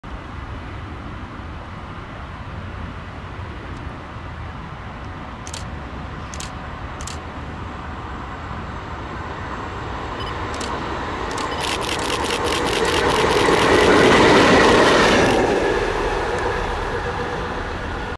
Crew 2454 C9002 Mat 24 komt door station Hollandsche Rading (4K)
Crew 2454 C9002 Mat 24 komt door station Hollandsche Rading onderweg naar richting Utrecht Centraal  (4K)